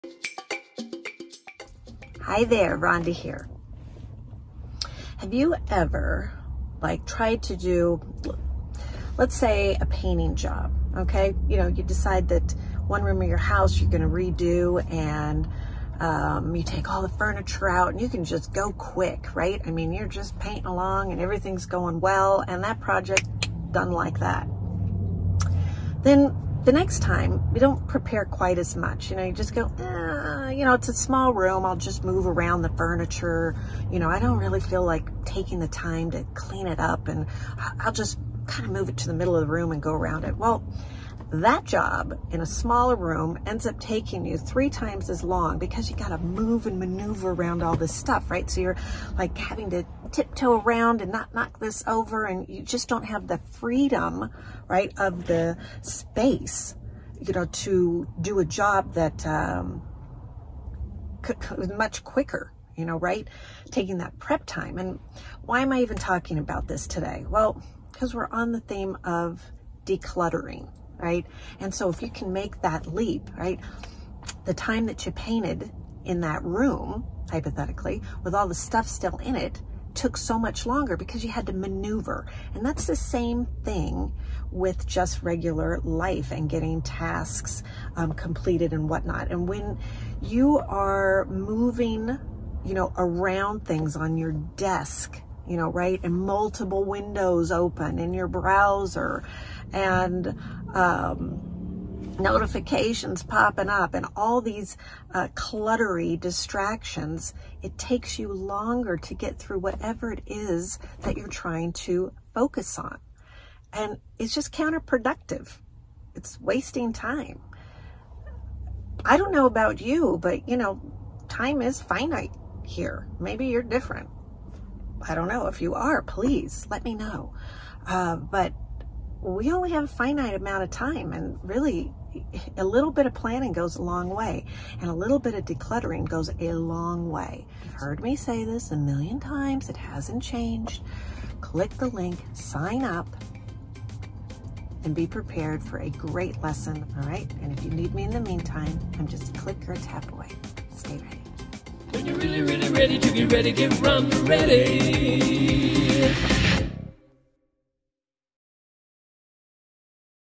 May 2022 UNversity trailer 3
Ronda shares a few more thoughts about de-cluttering. Because her next UNversity class is about how to De-Clutter. It's today, May (5/5/2022) Here's the link, to enroll, mentioned in the video
(this extended enrollment opportunity is rare, so don't miss out)